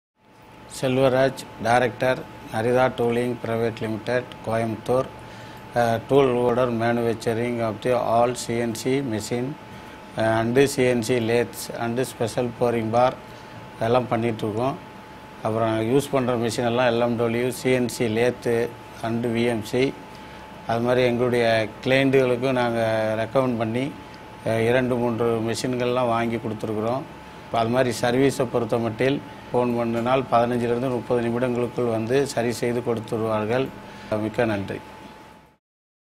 Testimony of Mr. Selvaraj - Director, NARITHA TOOLINGS
Mr. Selvaraj - Director, NARITHA TOOLINGS
speaks of his association with LMW and personally recommends LMW machines for high precision.